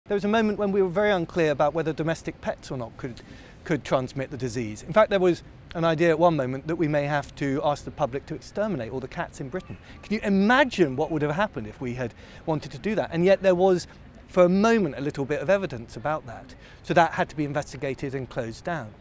Former Health Minister Lord Bethell discusses the early government response to the Covid pandemic.
"There was an idea at one moment that we may have to ask the public to exterminate all the cats in Britain."